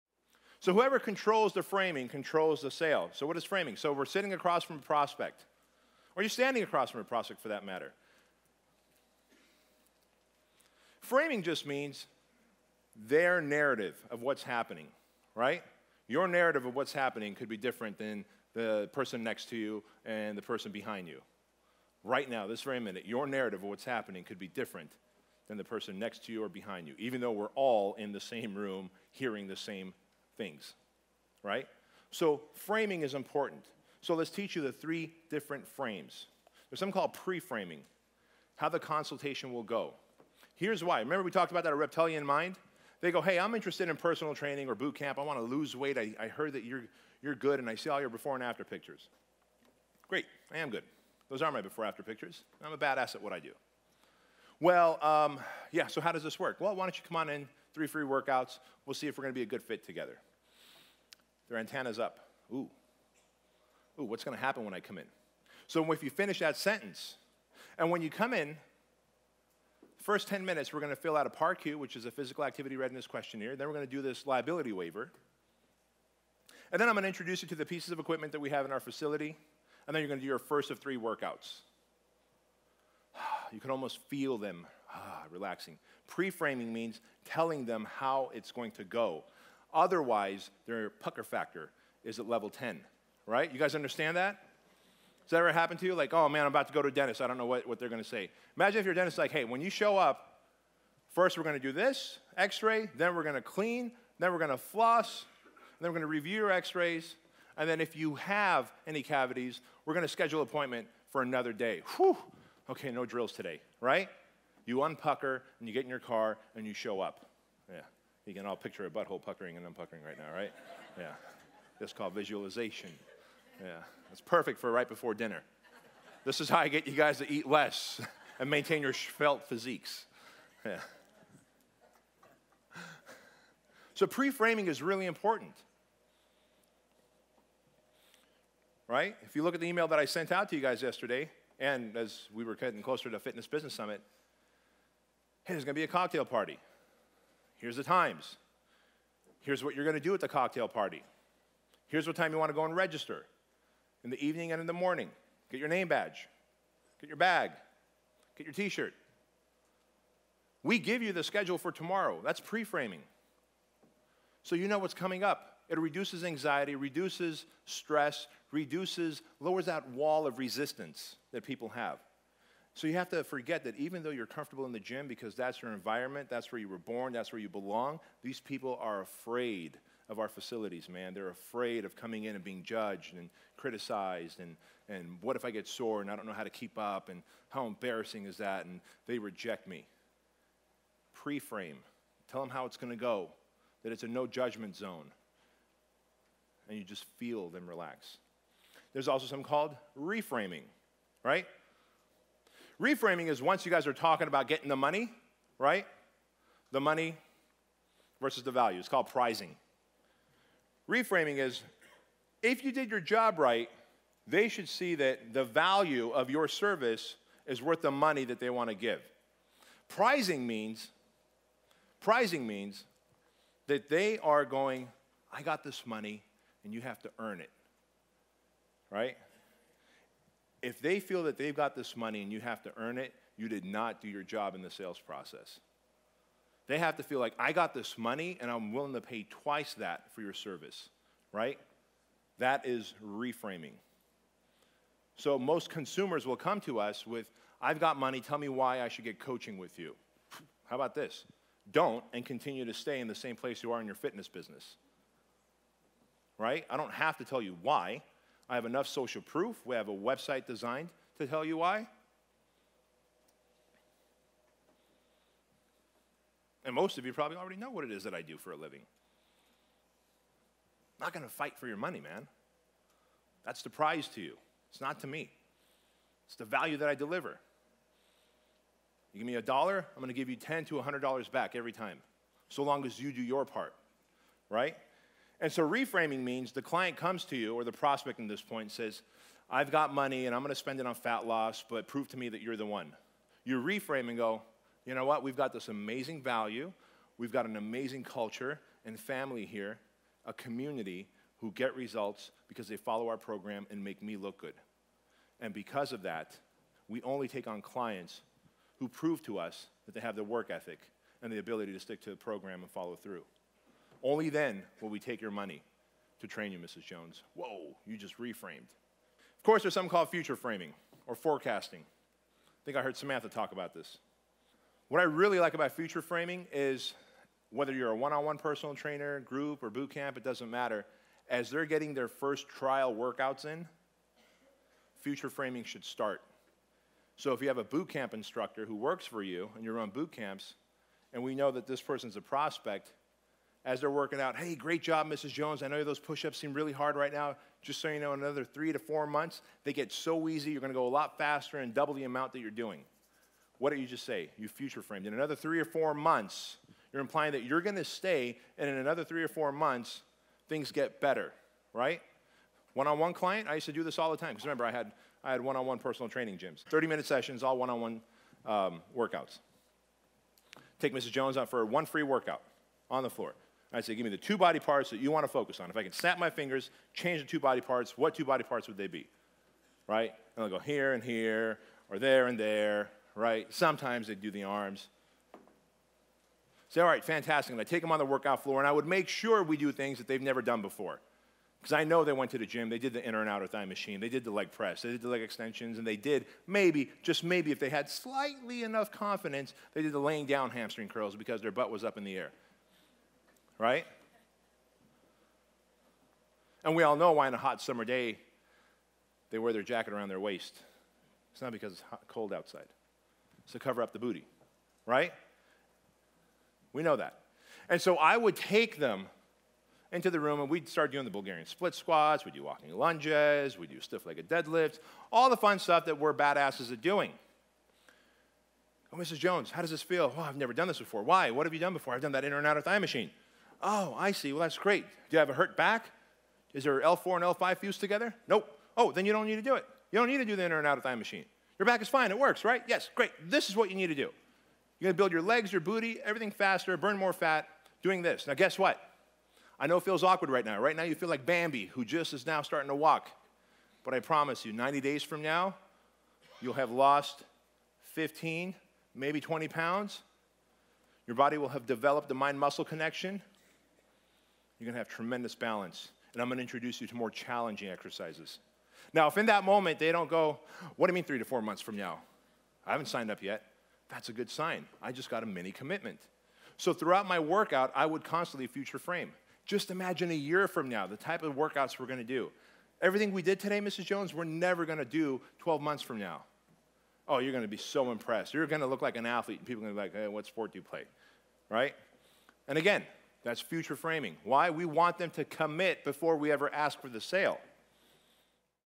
Whoever Controls The Framing, Controls The Sales
presents: Whoever Controls The Framing, Controls The Sales.  Bedros Keuilian, founder and CEO of Fit Body Boot camp, discusses the value of guiding the narrative to stay on top of clients' expectations.  Hear how this simple technique can help you acquire new clients with a better understanding of what they're signing up for.